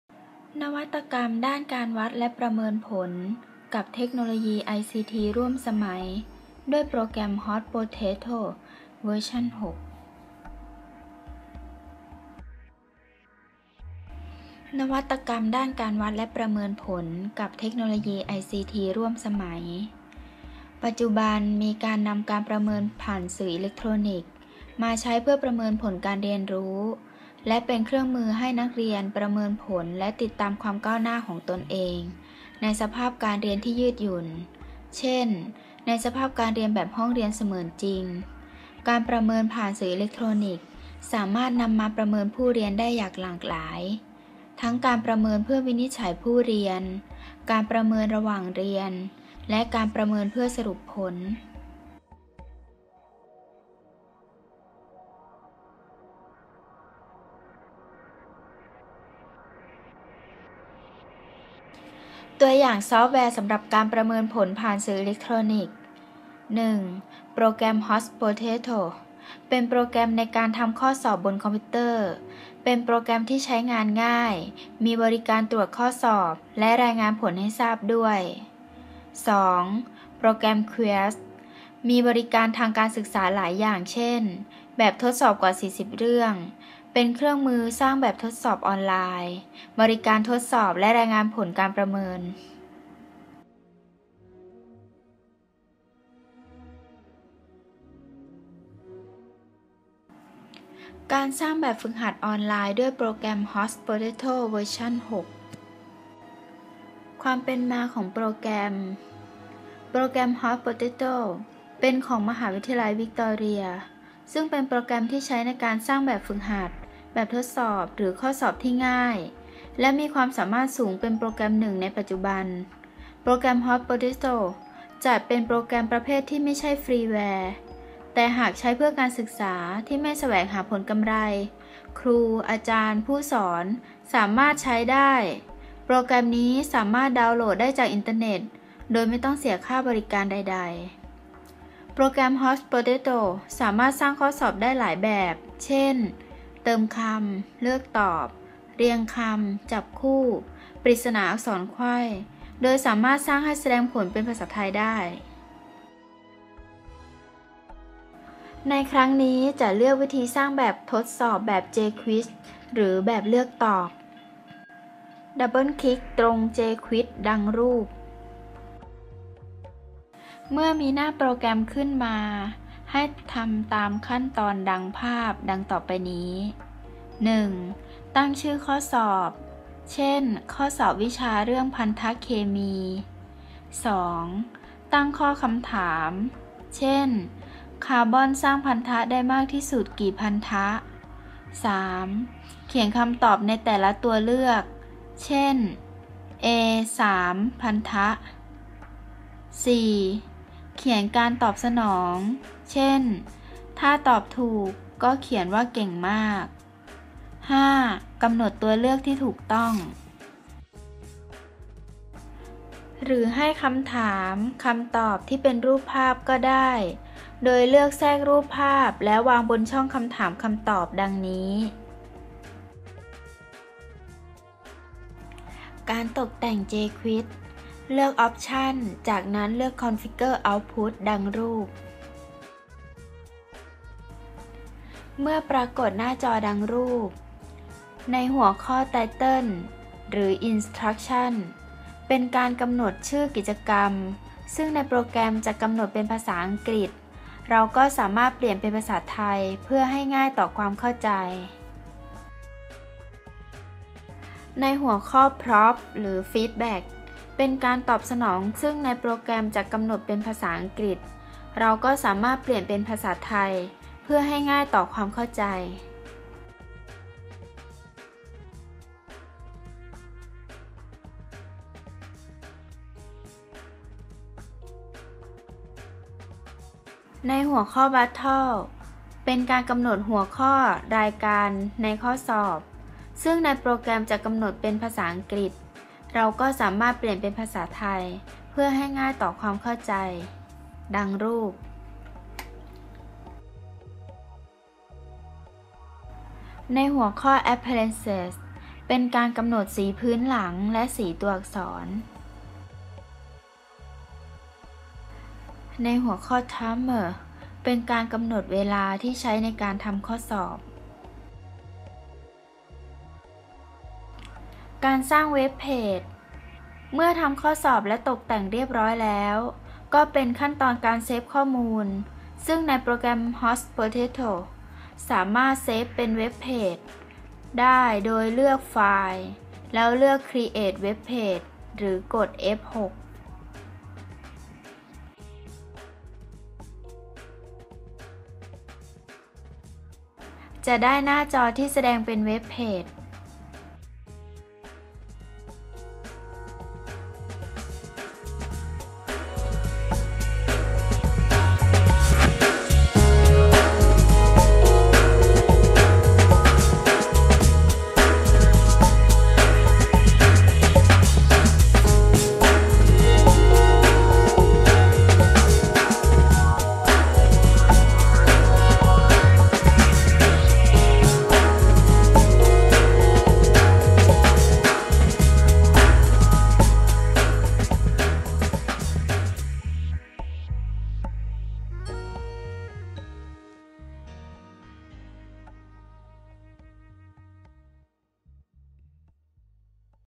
Hot Potato Version 6
นวัตกรรมด้านการวัดและประเมินผลกับเทคโนโลยีไอซีทีร่วมสมัย ด้วยโปรแกรม Hot Potato Version 6